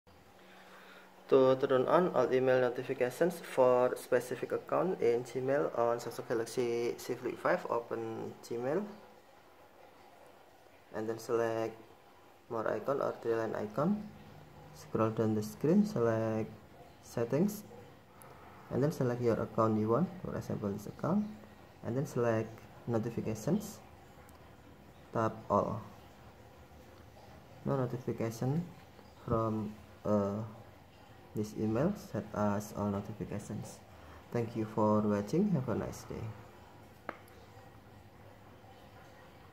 How to turn on All Email notifications for specific account in Gmail on Android (Samsung Z Flip5)
Email notifications play a crucial role in staying updated and responsive to incoming messages. If you're a Samsung Galaxy Z Flip5 user managing multiple email accounts through Gmail, ensuring that you receive notifications for all emails in a specific account is essential. This guide will walk you through the steps to turn on all email notifications for a specific account on your Samsung Galaxy Z Flip5.

**Step-by-Step Guide:**
1. **Launch Gmail App:** Unlock your Samsung Galaxy Z Flip5 and open the Gmail app.
2. **Access Settings:** Tap on the menu icon (three horizontal lines) located at the top left corner of the screen to open the navigation drawer. Scroll down and select "Settings."
3. **Choose Account:** If you have multiple accounts linked to the Gmail app, select the specific account for which you want to turn on email notifications.
4. **Navigate to Notifications Settings:** In the Settings menu, tap on the selected account to access its notification settings.
5. **Select Notifications:** Look for the "Notifications" option and tap on it to access email notification settings for the chosen account.
6. **Enable Notifications:** Toggle the switch next to "Email notifications" or similar wording to turn on notifications for all emails in the selected account.
7. **Adjust Additional Settings (Optional):** Some devices may offer additional options to customize notification settings further. If available, adjust these settings according to your preferences.
8. **Save Changes:** After enabling notifications, tap the back arrow or navigate back to the previous screen. Gmail will automatically save your changes.

**Benefits of Turning On All Email Notifications for Specific Accounts:**
- **Stay Updated:** By receiving notifications for all emails in a specific account, you can stay informed and responsive to incoming messages.
- **Enhanced Communication:** Ensuring that you receive notifications for all emails allows you to maintain timely communication with contacts and respond promptly to important messages.
- **Improved Productivity:** Having access to notifications for all emails helps you prioritize tasks and manage your time more effectively.

**Considerations:**
- **Notification Overload:** Be mindful of enabling notifications for all emails, as this may result in a high volume of notifications and potential distractions. Consider your email usage habits and adjust settings accordingly.
- **Battery Life:** Constantly receiving notifications for all emails may impact battery life. Monitor your device's battery usage and adjust notification settings as needed to optimize battery performance.

**Conclusion:**
Customizing email notifications in Gmail on your Samsung Galaxy Z Flip5 allows you to tailor your inbox experience to suit your needs and preferences. By turning on all email notifications for specific accounts, you can stay informed and responsive to incoming messages, enhancing your overall communication experience.